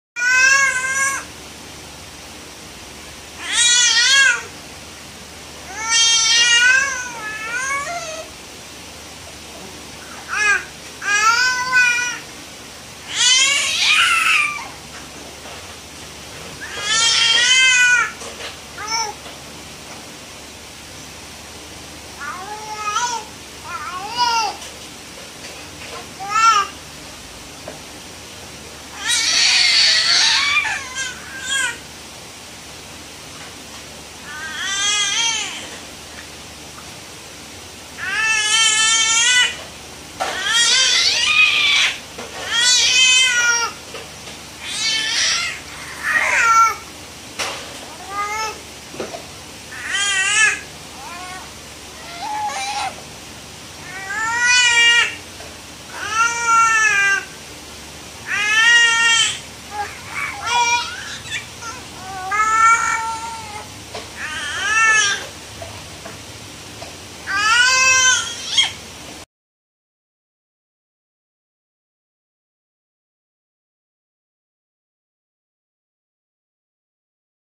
Caden and Caleb teether time
Caleb was just cool, but Caden.. I think there's a problem. 😂

Caden👶 & Caleb👶